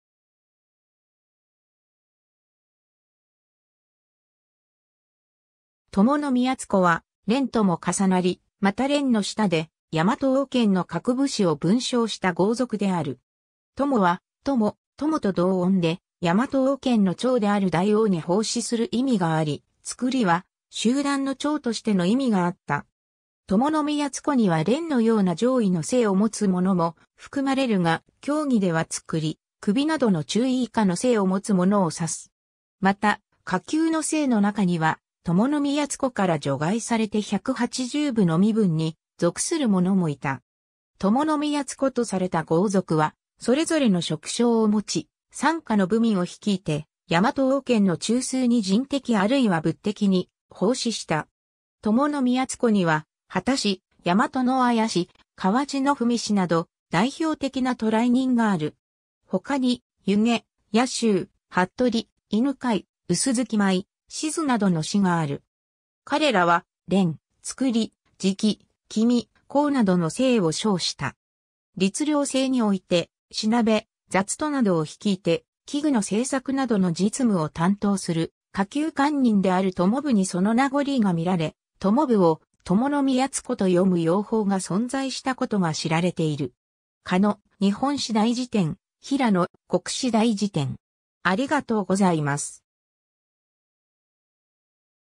伴造
伴造（とものみやつこ）は、連（むらじ）とも重なり、また連の下でヤマト王権の各部司を分掌した豪族である。
 「伴」は友・供と同音でヤマト王権の長である大王（おおきみ）に奉仕する意味があり、「造」は集団の長としての意味があった。
伴造には連のような上位の姓（かばね）を持つ者も含まれるが、狭義では造・首（おびと）などの中位以下の姓を持つ者を指す。
また、下級の姓の中には伴造から除外されて百八十部の身分に属する者もいた。
 伴造とされた豪族はそれぞれの職掌を持ち、傘下の部民を率いてヤマト王権の中枢に人的あるいは物的に奉仕した。
 伴造には、秦氏（はた）、東漢氏（やまとのあや）、西文氏（かわちのふみ）など代表的な渡来人がある。
他に、弓削（ゆげ）、矢集（やずめ）、服部（はとり）、犬養（いぬかい）、舂米（つきしね）、倭文（しとり）などの氏（うじ）がある。
彼らは、連、造（みやつこ）、直（あたい）、君・公（きみ）などの姓（かばね）を称した。
 律令制において品部・雑戸などを率いて器具の製作などの実務を担当する下級官人である伴部にその名残が見られ、伴部を「とものみやつこ」と読む用法が存在したことが知られている。
 ^ 狩野『日本史大事典』 ^ 平野『国史大辞典』